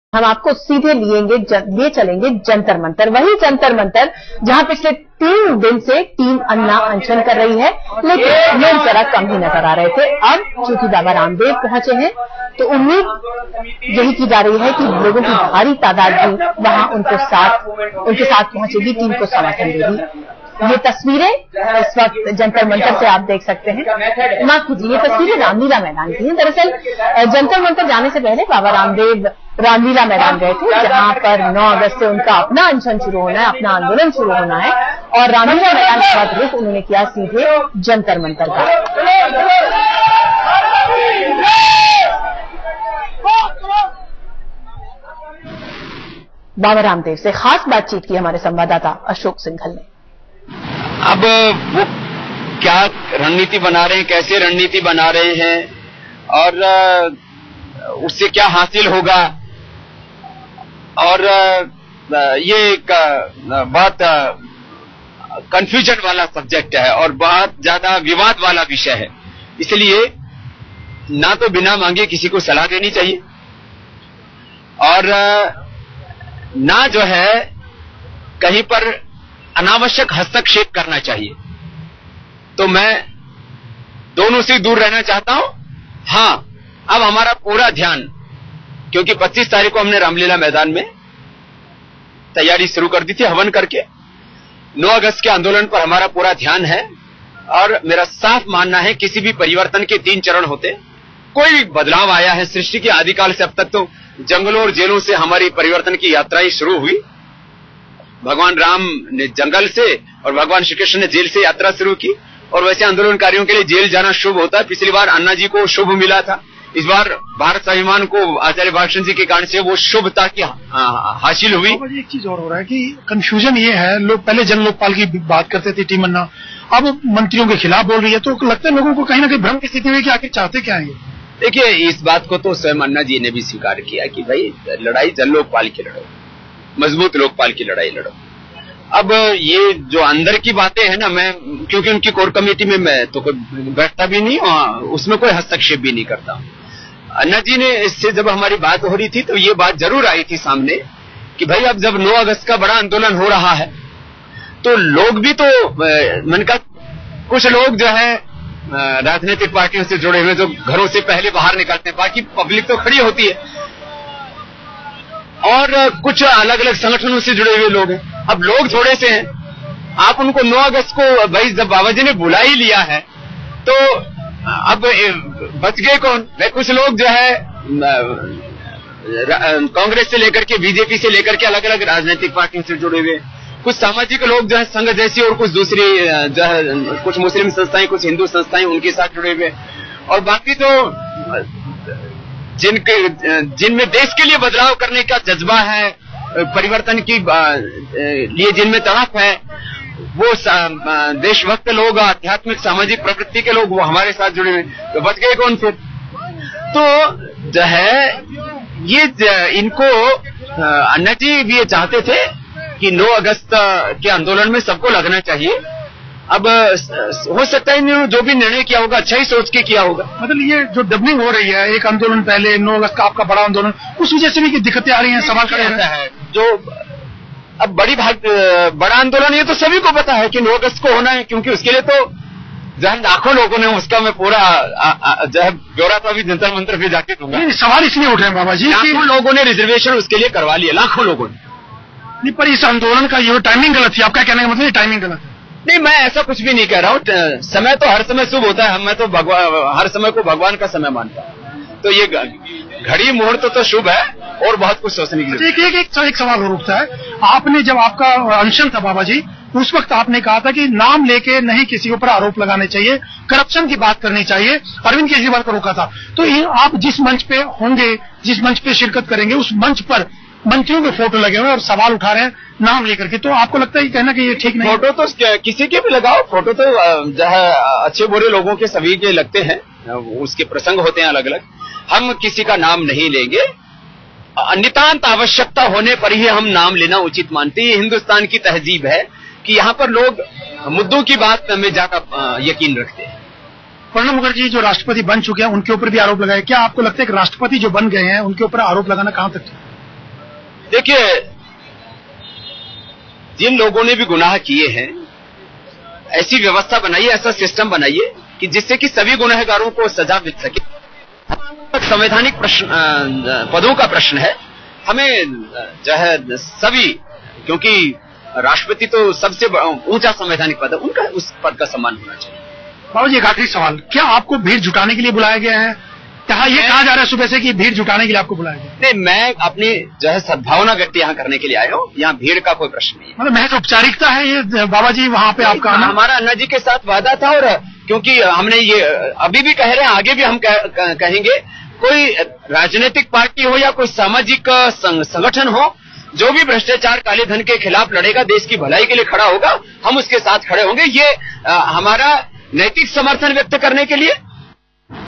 टीम अन्ना का प्रणब पर
वैसे तो टीम अन्ना के आंदोलन को रामदेव का साथ मिल गया है लेकिन इशारों इशारों में रामदेव ने अन्ना के अनशन पर सवाल उठा दिया है. उन्होंने कहा है कि प्रणब मुखर्जी और अन्य मंत्रियों पर टीम अन्ना द्वारा निशाना साधना गलत है